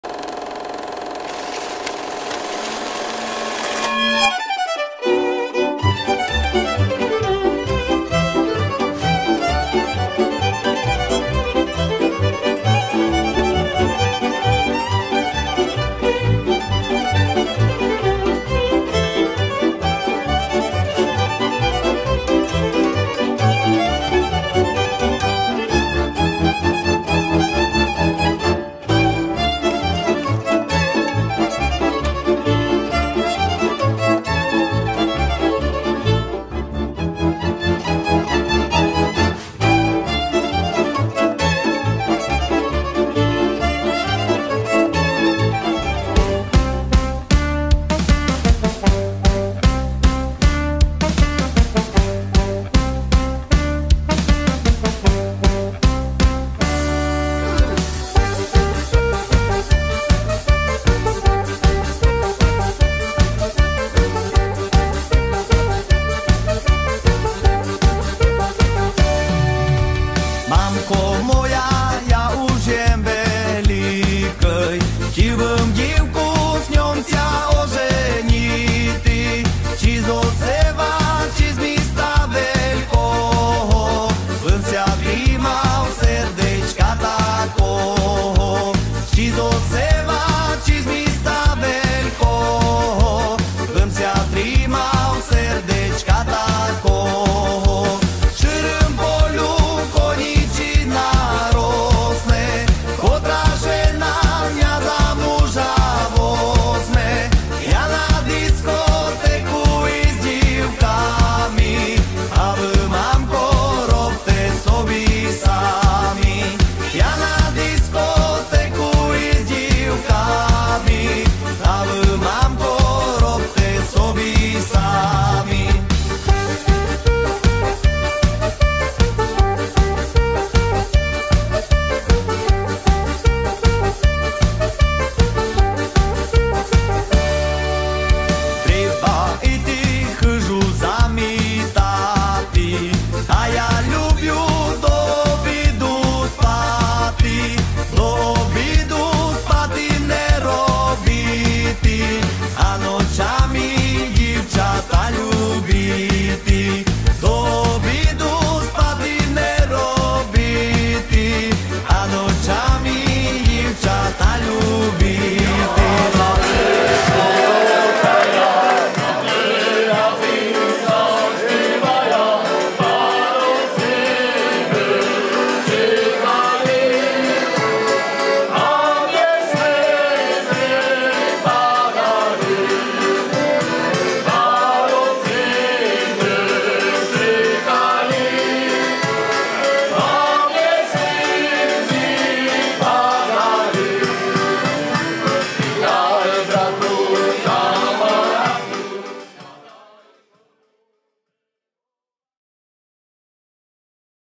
Obecná zabíjačka a Kobylská kolbasiada v Kobylách (14.1.2023)
Video z obecnej Zabijačky a nasledne z 10. ročníku Kobylskej Kolbasiady v Kobylách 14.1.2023

OBEC KOBYLY / PILSI_PRODUCTION ©